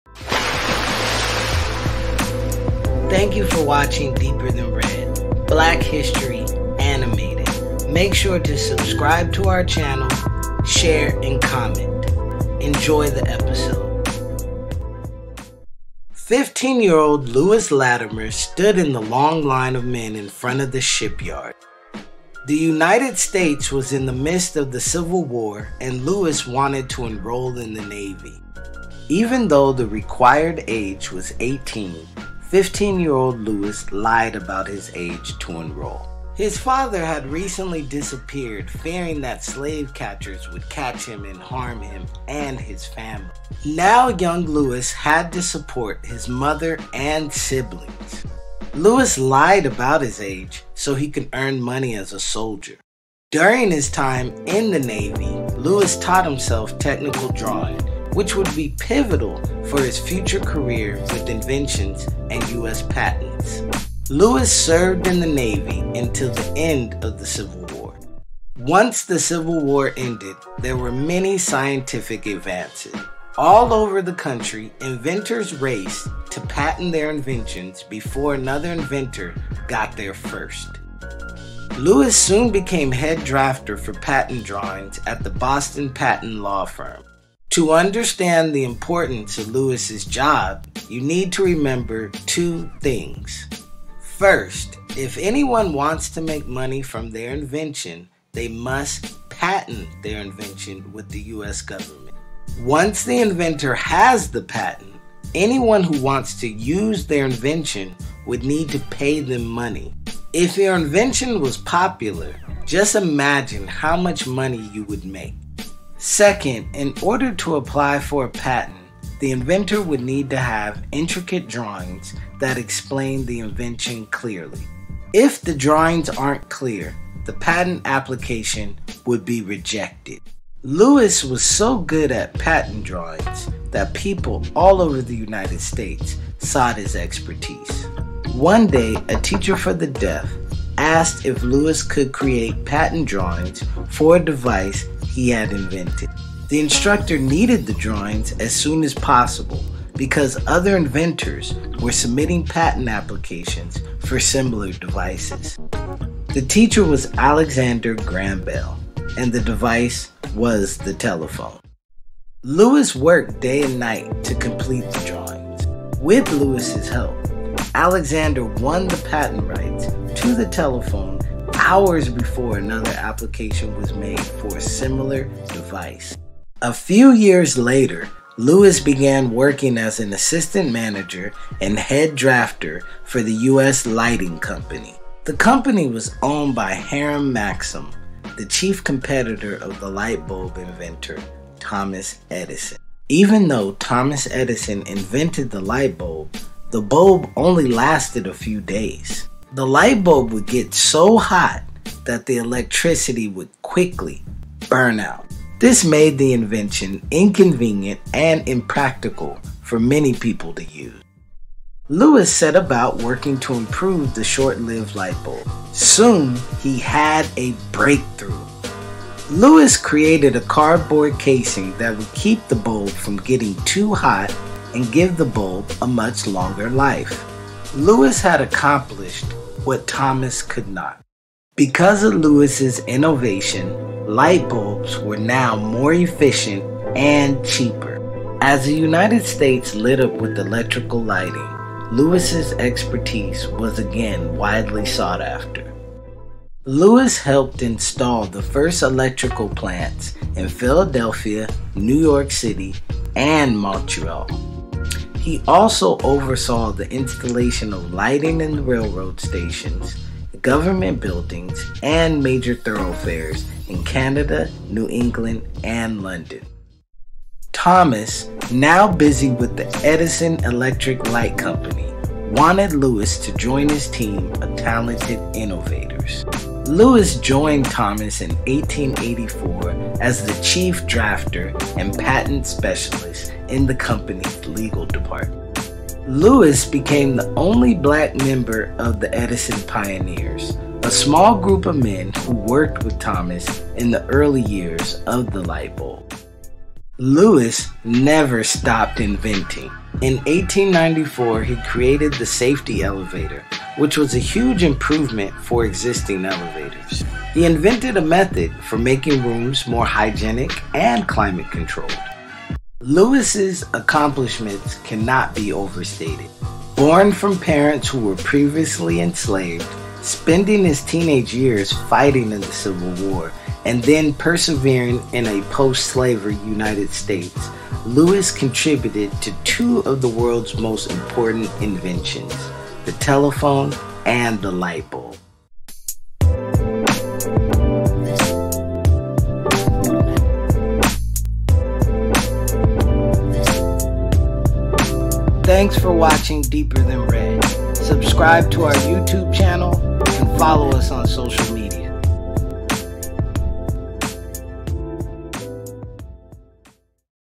Lewis Latimer Black Inventor.Lewis Latimer Black History Heroes.Deeper Than Read (Ep.29)💡📞⚙️
deeperthanread takes African-American History and re-imagines the subject in a new animated way! Dive In!